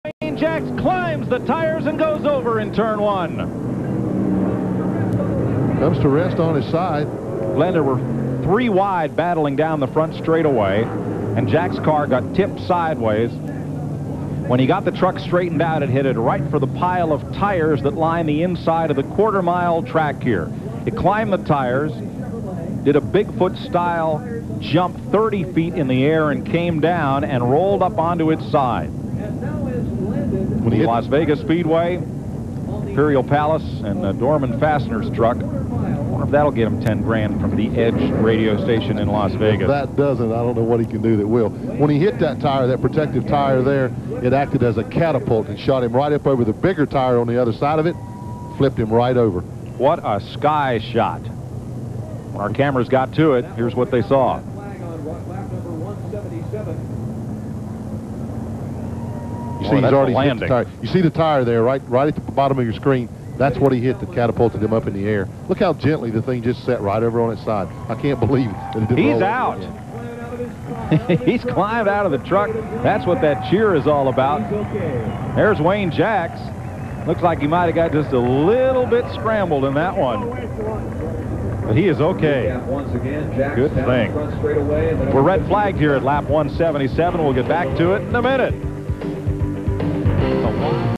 1995 Wayne Jacks flip @ Portland
Wayne Jacks flips during the 1995 Maxx Race Cards 200 NASCAR SuperTruck Series by Craftsman race at Portland.